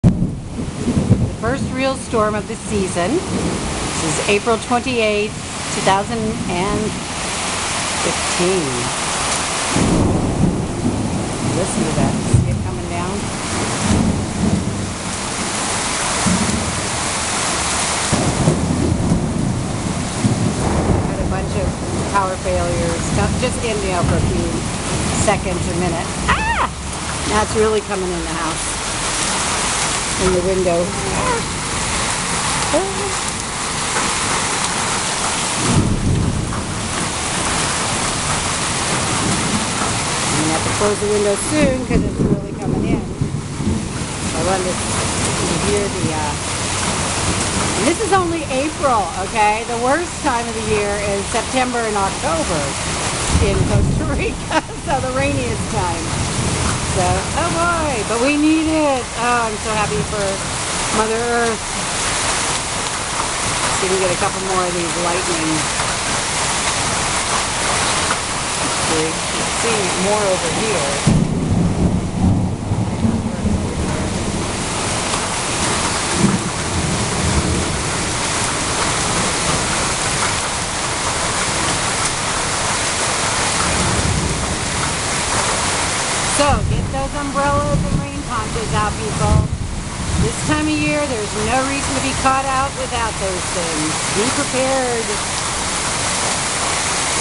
1st storm of the 2015 Rainy Season in Escazu, Costa Rica
1st real RAIN storm of the 2015 Rainy Season (at least in Escazu near the Country Club - at my base).
MY observation in my coming on 10 years (July 19) living in Costa Rica is the Rainy Season doesn't really get going until around late May-ish/June-ish till around November/December-ish with the heaviest rains around September-October (at least for the ESCAZU•Central Valley•San Jose area).
We had HAIL literally the size of 500-colon • 50¢-US$ pieces & 3/4-1" thick (didn't have my camera working for that).

Note it only lasted about 20 minutes.